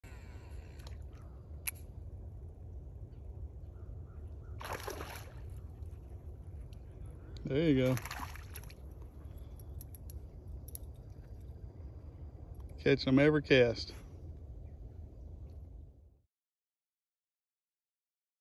Took my son to the creek for some white bass fishing.
Took my son to the creek for some white bass fishing. Most were small, but the bigger ones will be there soon.